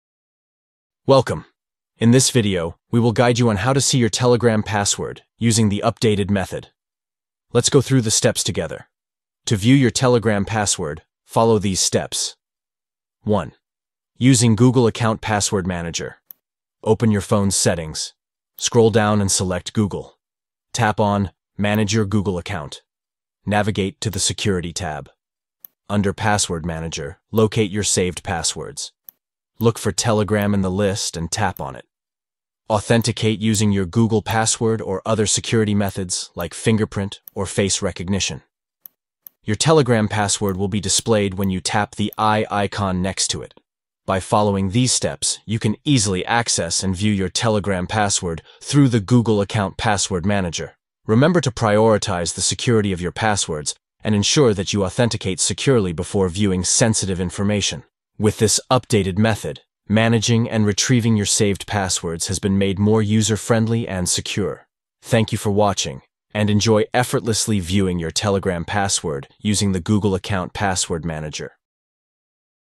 How To See Your Telegram Password (Updated)
Learn how to see your Telegram password with the latest updates. Follow these steps to retrieve your Telegram password using Google Account Password Manager:

1. Using Google Account Password Manager:
2. Open your phone’s Settings.
3. Scroll down and select Google.
4. Tap on Manage your Google Account.
5. Navigate to the Security tab.
6. Under Password Manager, you’ll find your saved passwords.
7. Look for Telegram in the list and tap on it.
8. Authenticate using your Google password or other security methods (such as fingerprint or face recognition).
9. You’ll see your Telegram password displayed when you tap the “eye” icon next to it.

Discover how to access your Telegram password securely and effortlessly.